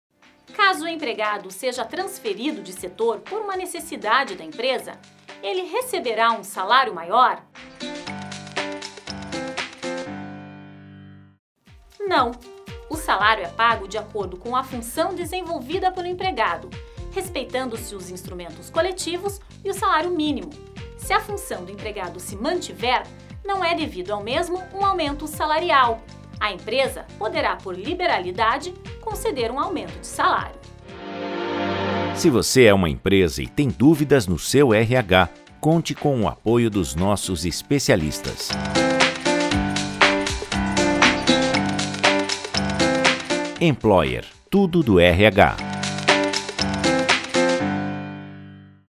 CASO O EMPREGADO SEJA TRANSFERIDO DE SETOR, ELE RECEBERÁ UM SALÁRIO MAIOR?
CASO O EMPREGADO SEJA TRANSFERIDO DE SETOR POR UMA NECESSIDADE DA EMPRESA, ELE RECEBERÁ UM SALÁRIO MAIOR?

Não.

Saiba todos os detalhes no vídeo acima.